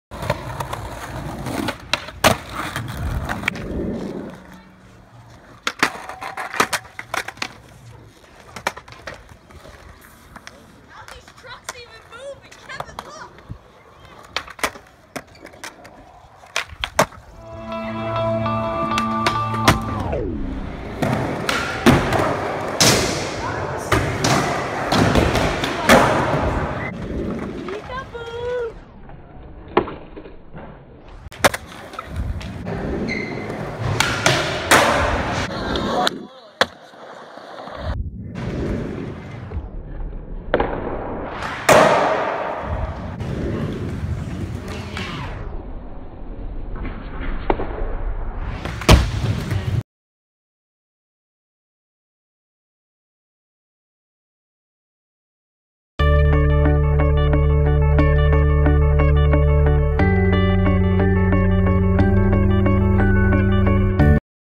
FIRST STREET HANDRAIL!!
Ty came through with some rad footage for the channel! He killed it and is constantly progressing! PEEL!